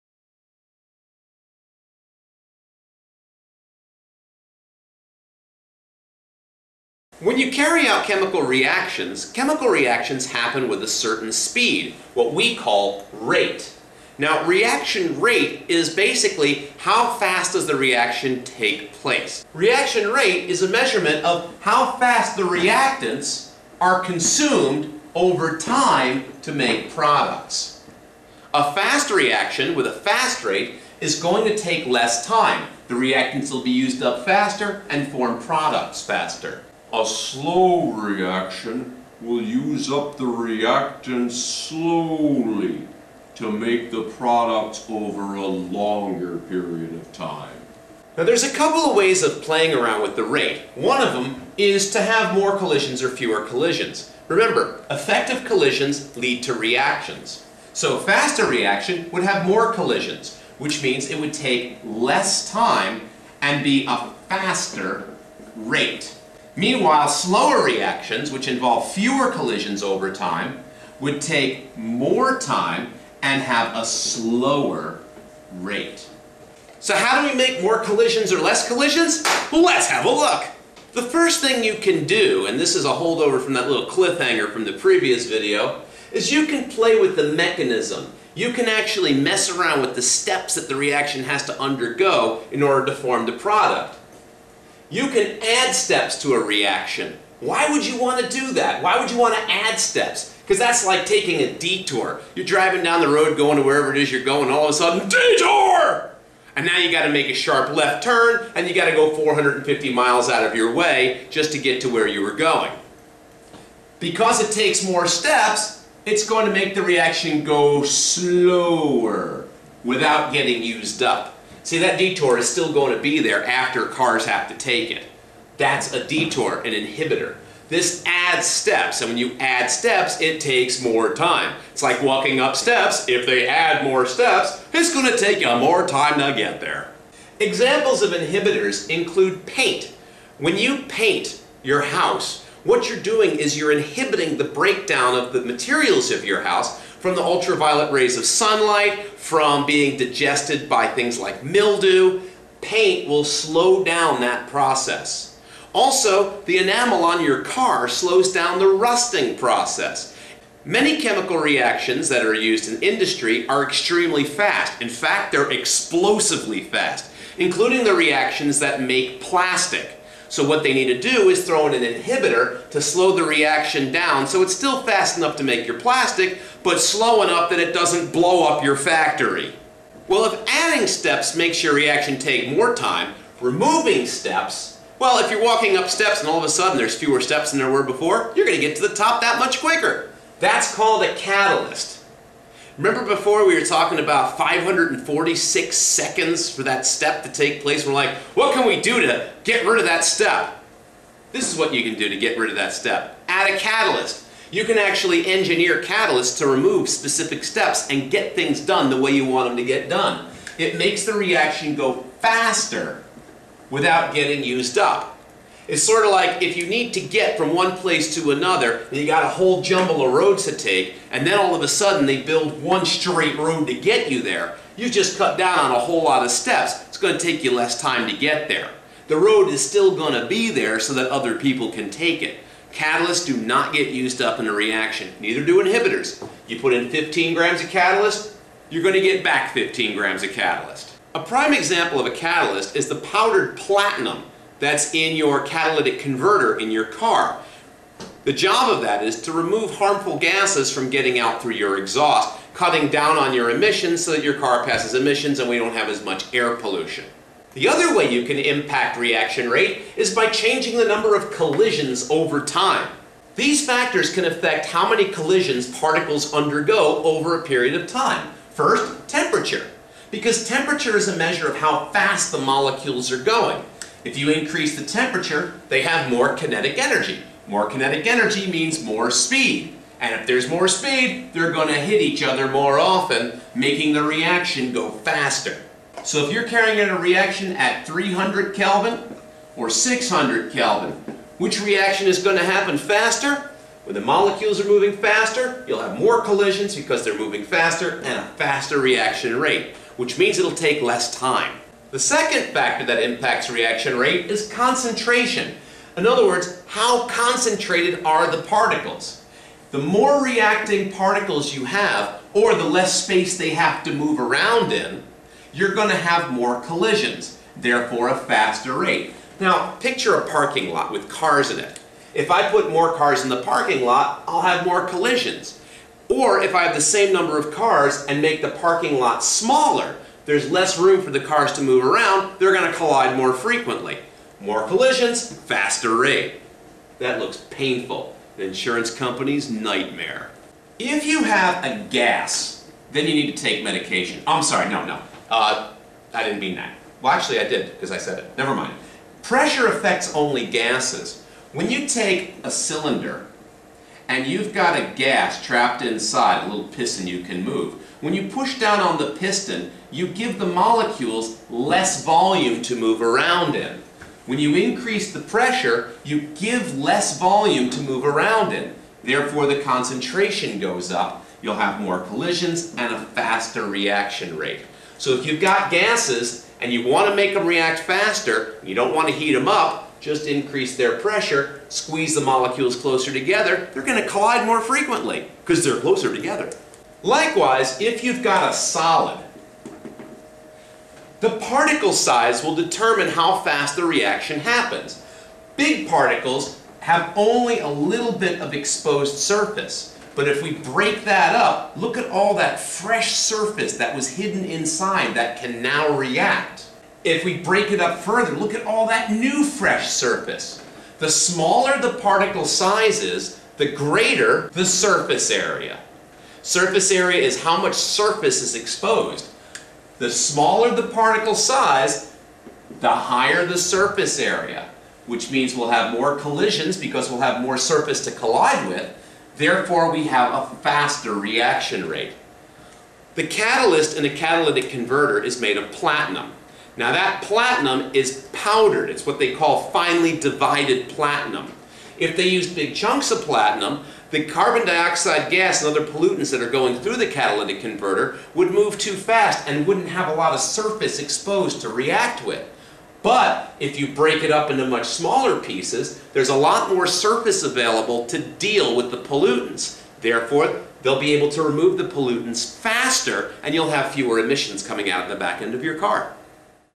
Chemistry Tutorial 9.01b: Factors Affecting Reaction Rate
How catalysts, inhibitors, temperature, pressure, concentration and surface area affect the rate of a chemical reaction.